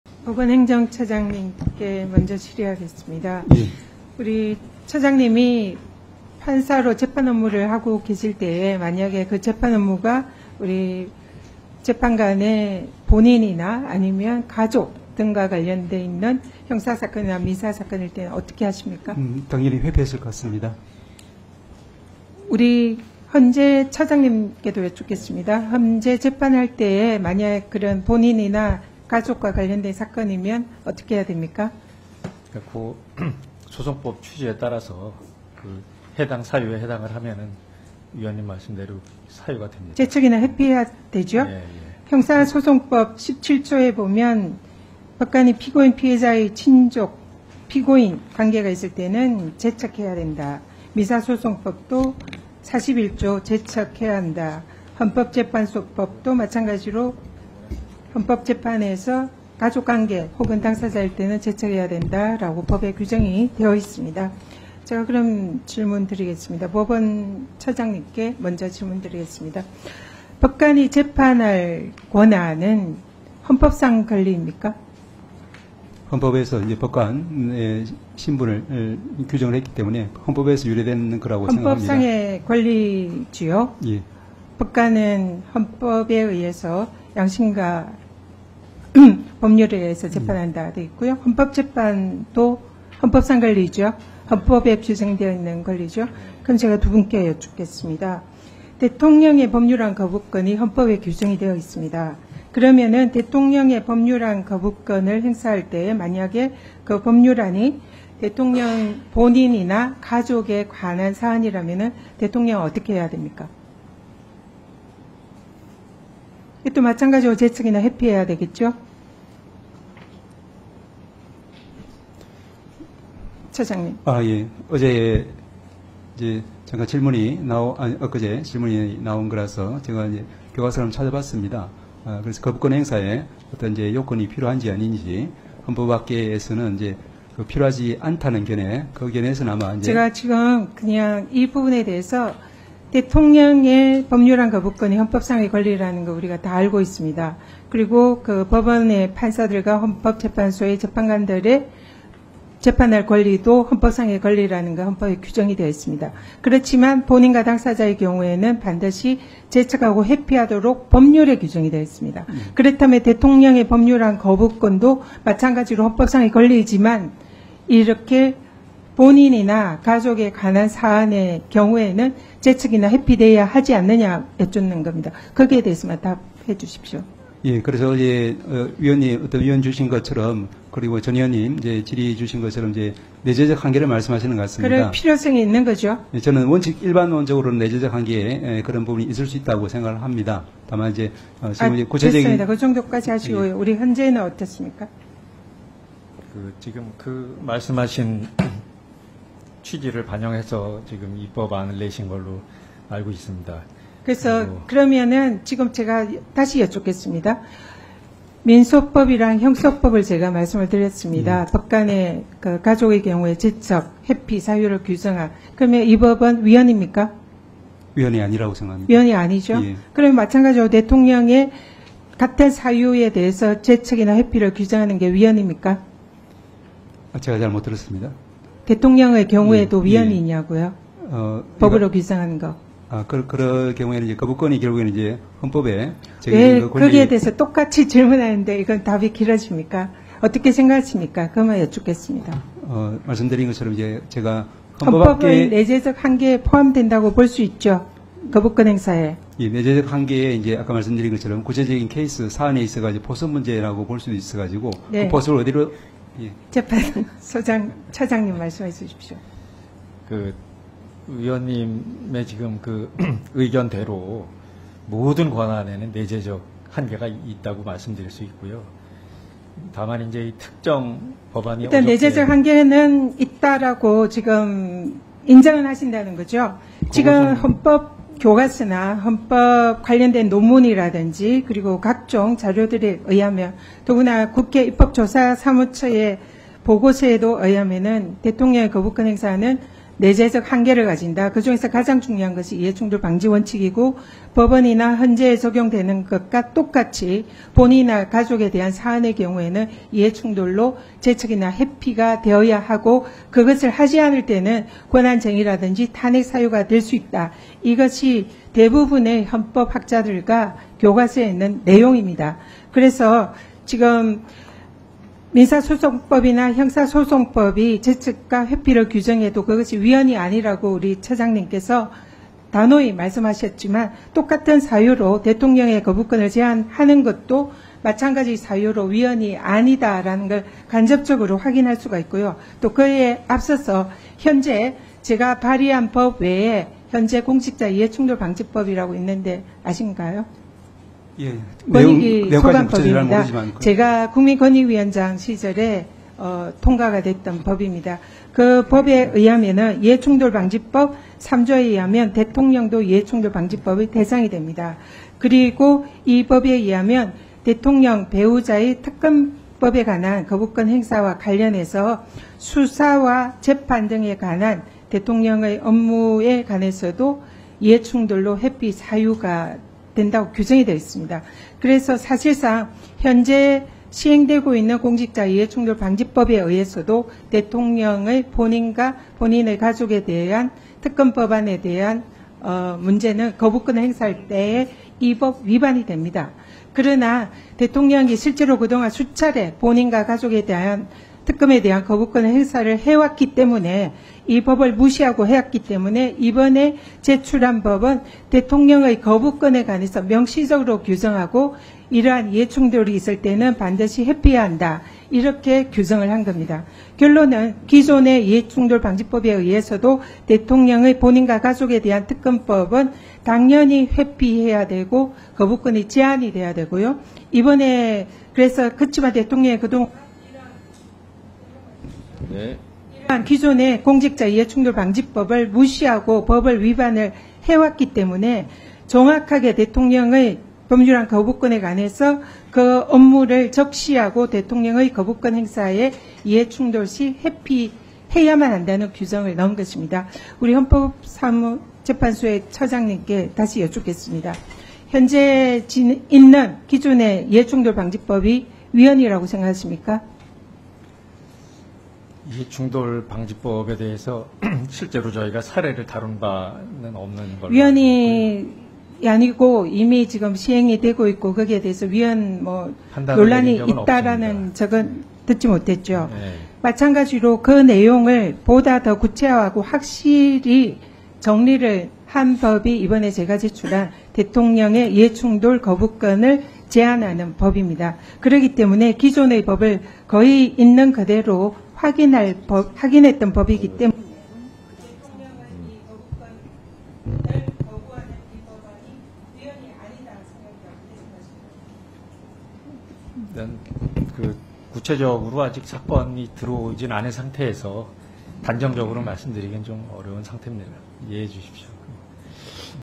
이걸 위한 ‘빌드업’일 줄이야... 전현희 “윤석열, 김건희 특검법 당연히 회피하는 게 맞네?”
14일 오후 국회 법사위 전체회의가 열렸다.

(영상 : 강보현 PD, 윤다림 PD)